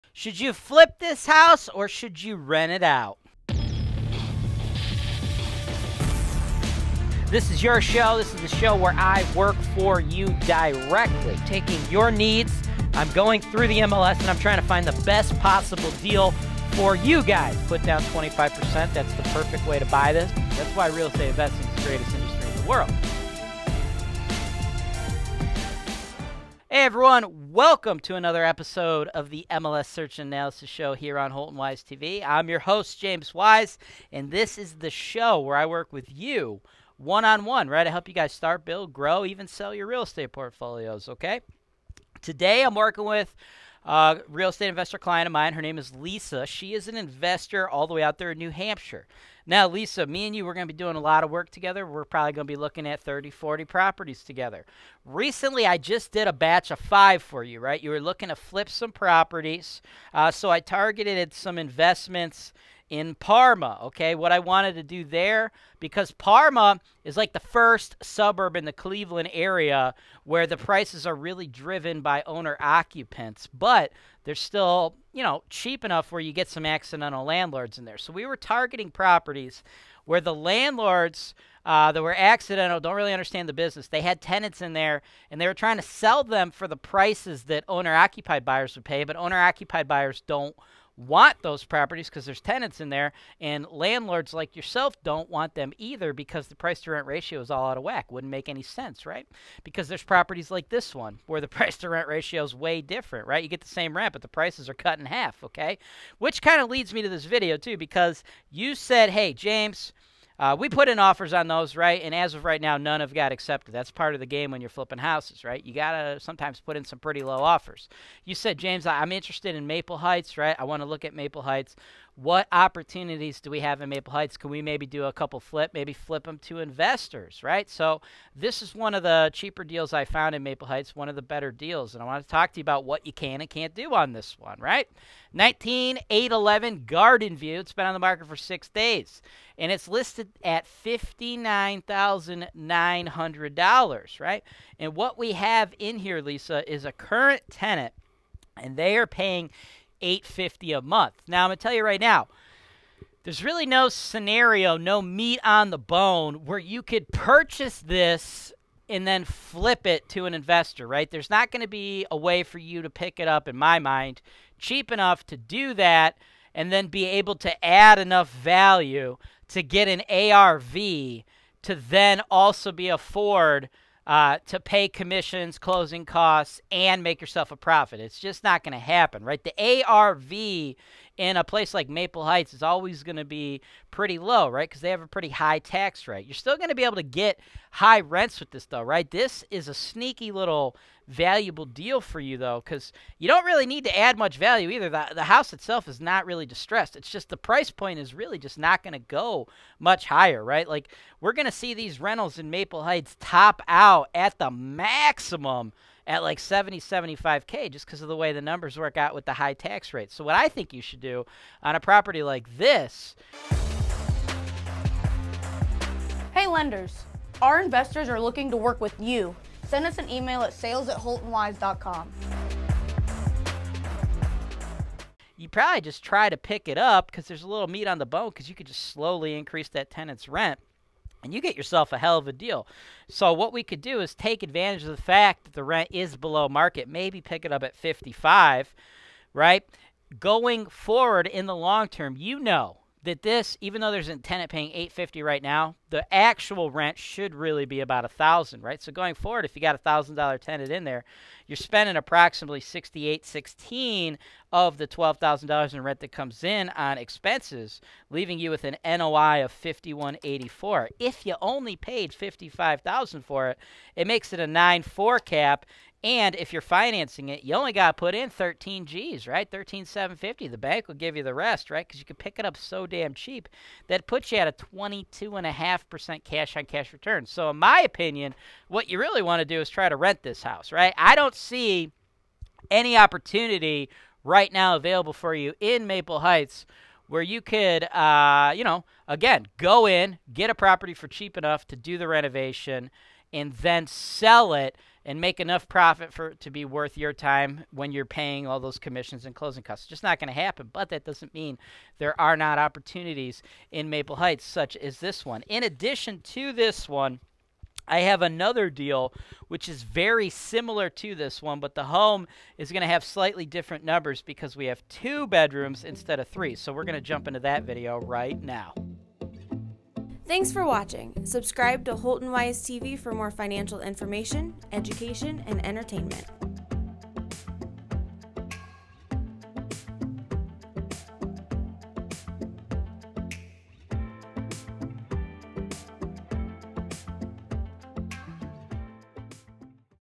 Rental Property Investing vs Flipping Houses | MLS Search & Analysis 395 - 19811 Gardenview
Perhaps you've read flipping houses for dummies and are considering flipping houses. Maybe you've thought about passive income and receiving mailbox money so you're thinking about rental property investing. In the 395th episode of The MLS Search & Analysis Show James Wise is working with an investor from New Hampshire to determine which real estate investment strategy is better for her.

📺 HELPFUL RESOURCES